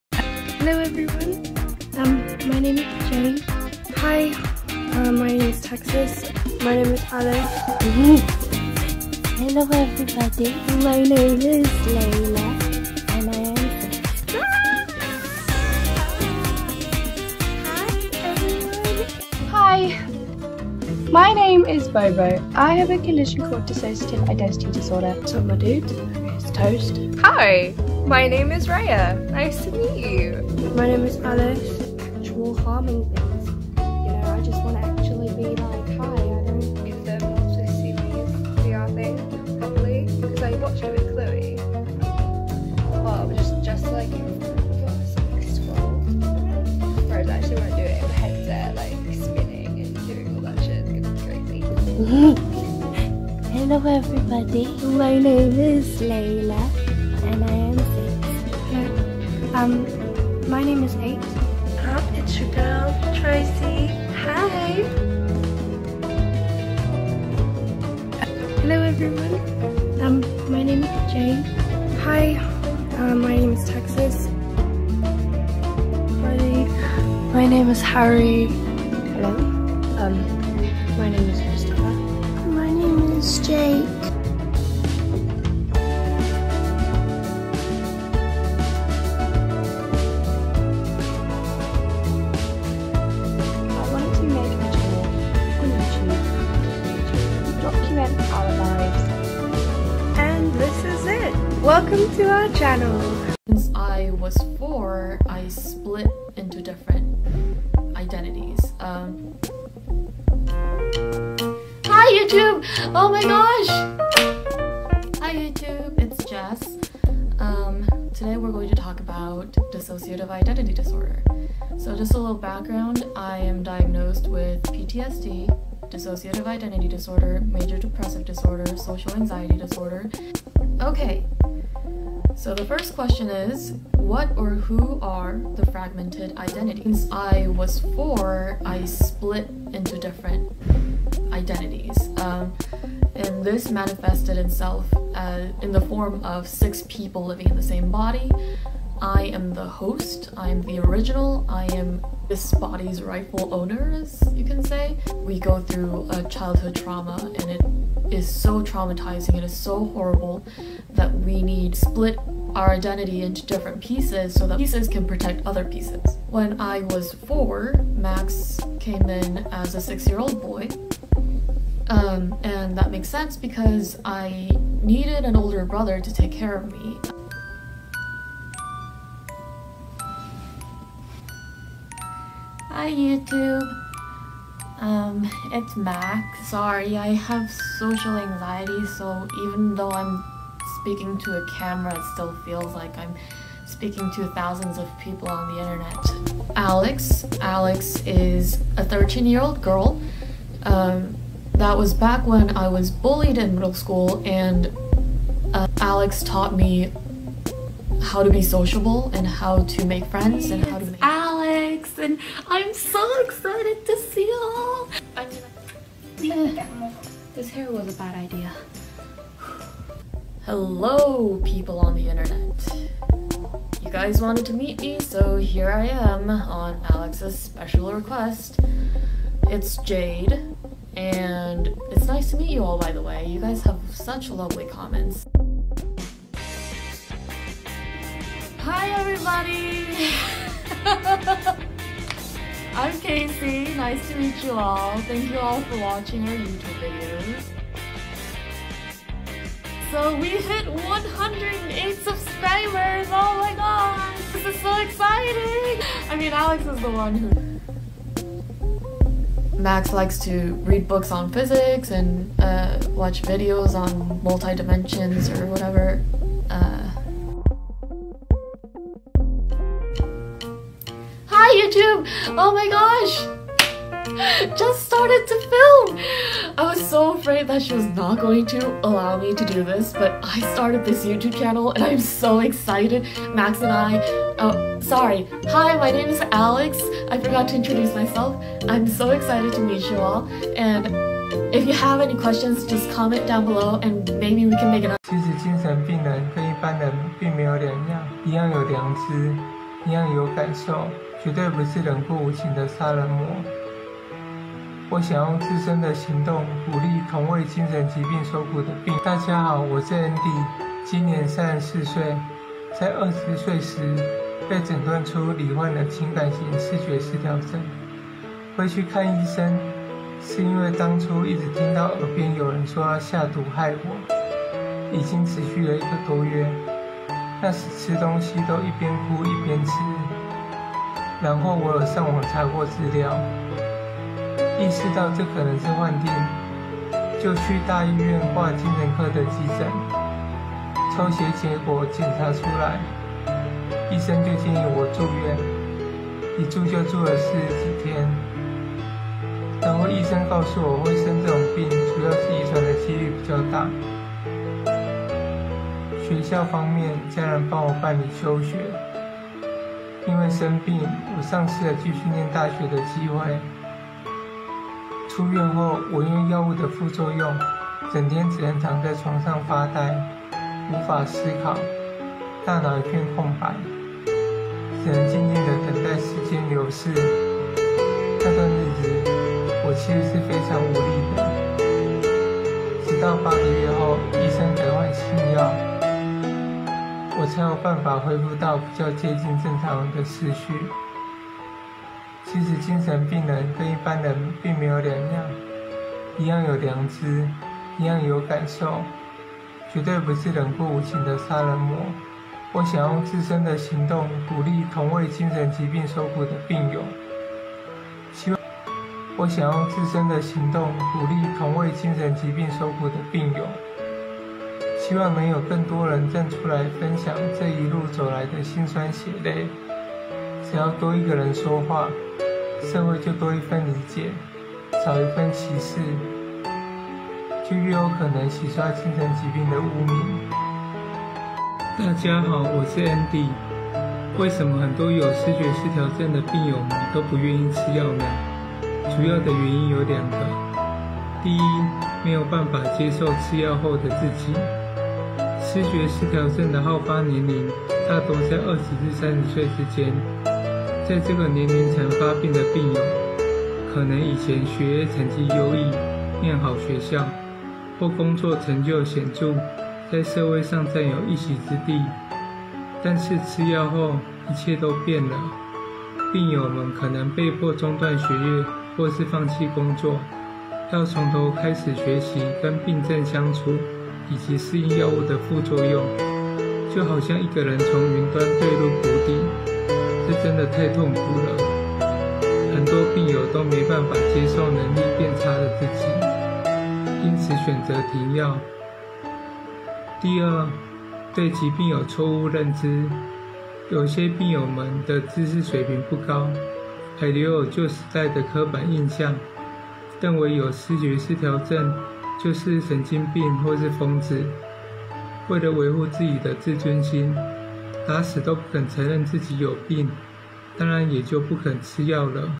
少女患病分裂25種人格！妖豔辣妹秒變6歲童
19歲少女擁有6種人格 講到一半「她」突然跑出
20歲被診斷思覺失調！他幻聽：有人毒害我

影片授權：AP／Caters TV MRSS
影片授權：YT／AlexMax Han
影片授權：Andy治癒家